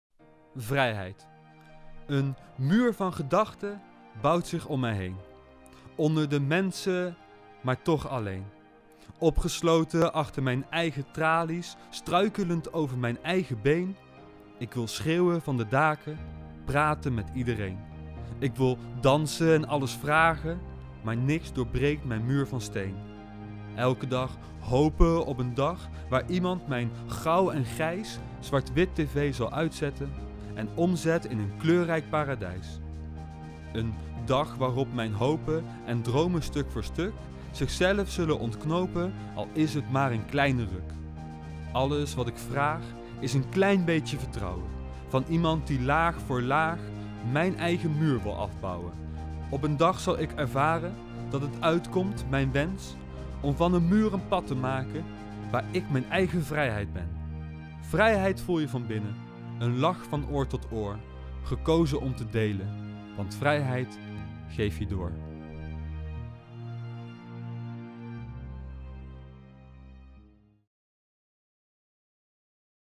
Vrijheid geef je door
We herdenken dit jaar thuis en samen. We staan stil bij onze vrijheid. Leerlingen van het Karel de Grote College hebben daar gedichten over geschreven. Vandaag hoor je het gedicht van Jasmijn.